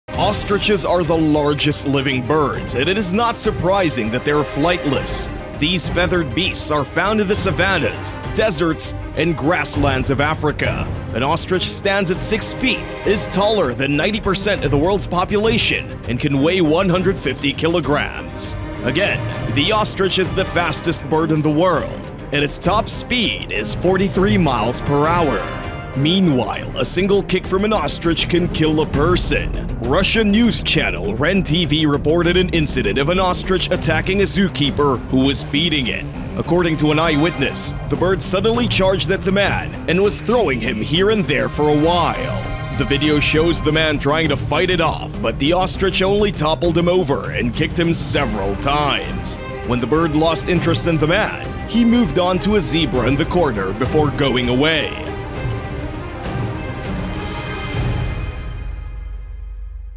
Most Dangerous Birds - Ostrich!
Ostriches are the largest living birds, and it is not surprising that they are flightless. These feathered beasts are found in the savannas, deserts, and grasslands of Africa. An ostrich stands at 6 feet, is taller than over 90% of the world's population, and can weigh 150kg. Again, the ostrich is the fastest bird in the world, and its top flight is 43 mph.
Meanwhile, a single kick from an ostrich can kill a person. They have four toes in total that are each 4 inches long. Indeed, they escape their predators by running or kicking them.
Russian News channel Ren TV reported an incident of an ostrich attacking a zookeeper who was feeding it. According to an eyewitness, the bird suddenly charged at the man and was throwing him here and there for a while. The video shows the man trying to fight it off, but the ostrich only toppled him over and kicked him several times. When the bird lost interest in the man, he moved on to a zebra in the corner before going away. The tourists were so surprised at how berserk the animal went suddenly. They watched with disbelief as the man who sustained injuries got up and picked his hat from the ground.